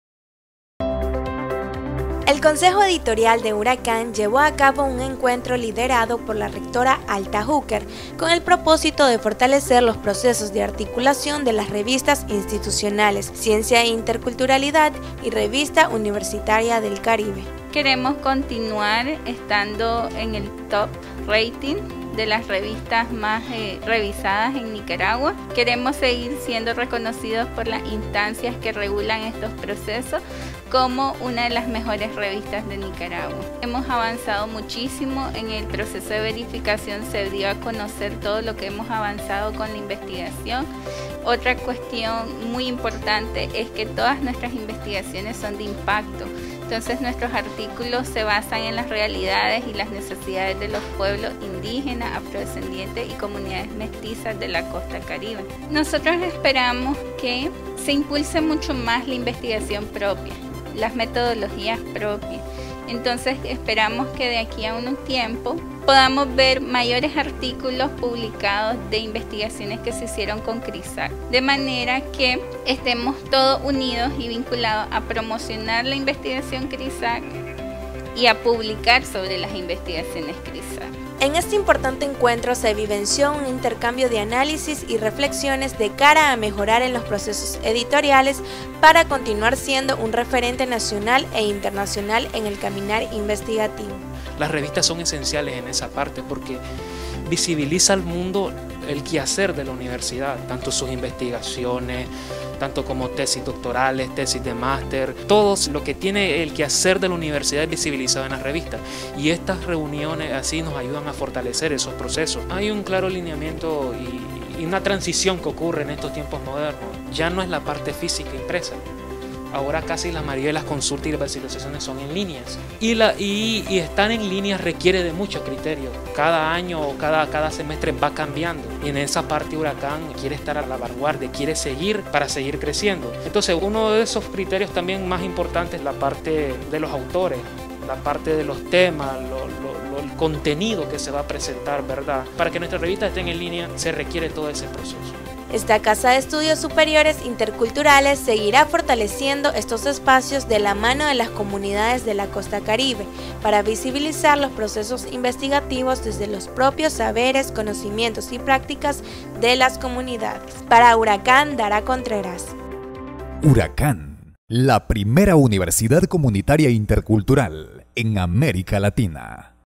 Consejo Editorial de URACCAN analiza la construcción y articulación de Revistas Institucionales
✒️📚 Miembros del Consejo Editorial de esta casa de estudios superiores interculturales, liderada por la magnífica rectora de URACCAN, Dra. Alta Hooker, sostuvieron un encuentro con el objetivo de analizar el proceso de construcción y articulación de las Revistas Institucionales; Ciencia e Interculturalidad y Revista Universitaria del Caribe.